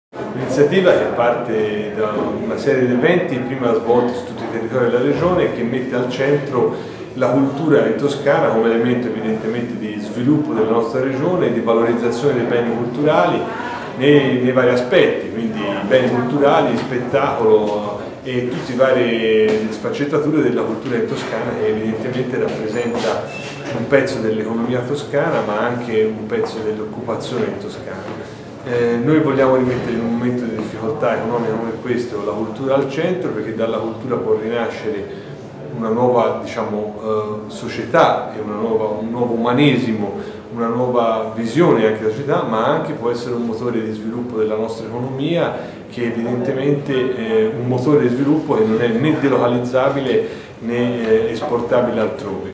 Nicola Danti: ''La cultura è un motore di sviluppo per il nostro Paese''
Il PD lancia gli Stati Generali della cultura in Toscana: sabato 19 novembre alla Casa della creatività di Firenze, dalle 9.30 alle 17.30, si confronteranno artisti, operatori del settore e amministratori, tra cui il presidente della commissione Cultura della Regione Nicola Danti